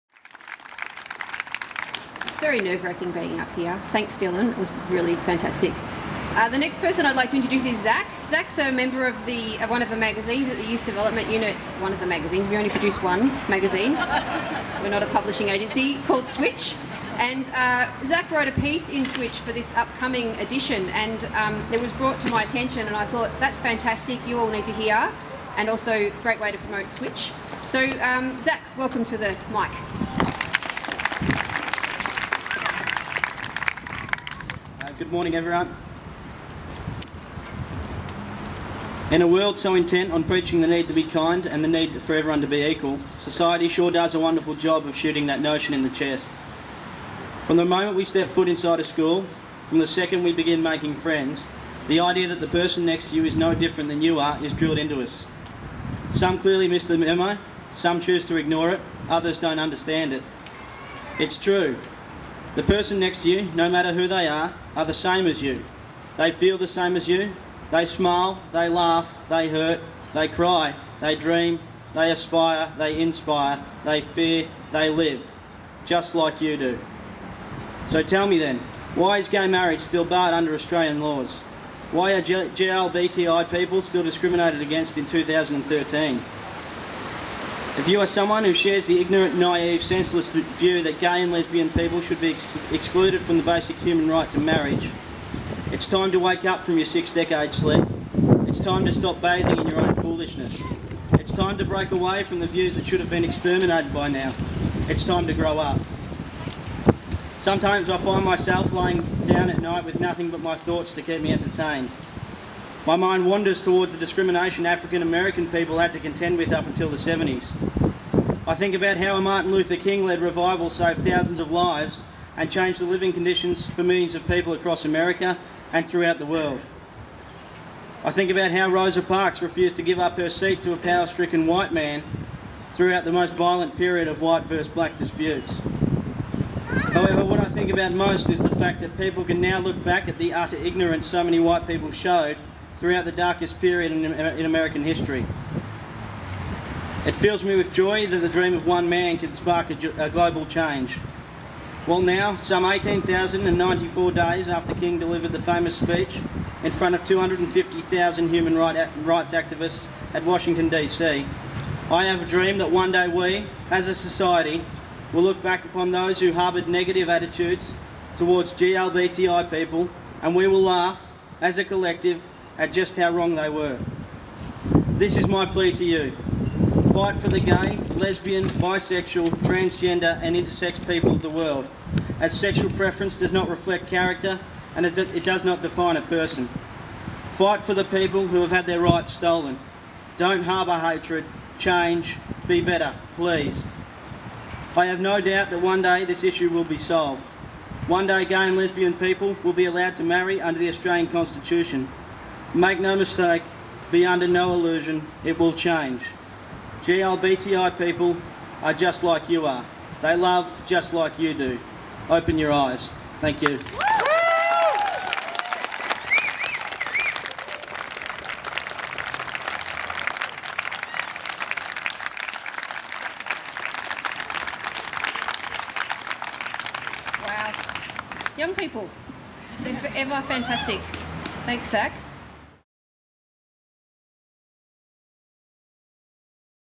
Gay Marriage Speech by Zakary Testa
The Geelong Australia celebration of IDAHO (International Day Against Homophobia and Transphobia) was celebrated with the rainbow flag raising ceremony at City Hall.

Local 18 year old Zakary Testa read aloud an article he wrote for local youth magazine 'Switch'.  This video is that entire speech. A powerful and thoughtful insight.

Geelong raises rainbow flag for IDAHO Day
Tuesday, 14 May 2013 10:52 AM Media Releases
The rainbow flag was raised at Johnstone Park today to celebrate the upcoming International Day Against Homophobia and Transphobia (IDAHO Day) on Friday 17 May.

The flag was raised by City of Greater Geelong Mayor Cr Keith Fagg and Youth portfolio holder Cr Jan Farrell. They were joined by many members of the Geelong community, both young and old. A number of young people spoke about why they became involved in the campaign and why their community must advocate to stamp out homophobia and transphobia.

The ceremony coincides with IDAHO Day on 17 May, which was created in 2004 to increase awareness of the issues of legal discriminations, social homophobia and transphobia.

This year for the first time IDAHO will be officially recognised internationally by the United Nations. A special video is set to be released by the United Nations agencies, featuring a statement from the UN Secretary General, Ban Ki-Moon

The rainbow flag is a symbol of solidarity and hope for the gay, lesbian bisexual, transgender and intersex (LGBTI) communities. Across Geelong, there will be several IDAHO celebrations with around 24 schools, community organisations and businesses taking part, supported by small grants from the City of Greater Geelong.

Mayor Keith Fagg said the City of Greater Geelong was proud to support the IDAHO message.

"By raising the rainbow flag we are making a bold and positive statement that Greater Geelong is an inclusive, diverse and welcoming community," he said.

"With 24 schools and organisations celebrating throughout the week it is clear that the Geelong community is committed to standing up against homophobia, transphobia and all forms of discrimination."

"Discrimination has no place in our community and the aim of today's flag-raising is to increase awareness of this issue and show our support for our LGBTI community," said the Mayor.

Youth portfolio holder Cr Jan Farrell said that the flag-raising ceremony was about celebrating diversity and making a positive statement about what Geelong represents.

"National and international research has shown a clear connection between discrimination and negative long term affects on an individual's health and wellbeing."

"The sad truth is that despite many changes in society, 70% of gay and lesbian young people around Australia are still struggling with homophobic abuse and discrimination."

"As a result of this discrimination around 37% of gay and lesbian young people have considered self harm. That's a really sad indictment on our society and how we collectively treat our GLBTI young people."

"I am very proud of the ongoing support that this Council provides to our same sex and gender questioning young people. Groups like gasp and events like this and the Gay Pride March are very important for their health and well being. Today's flag-raising is a public statement of support and a celebration of diversity in our community," said Cr Farrell.

For information about Geelong support services for gay, lesbian, bisexual or gender diverse young people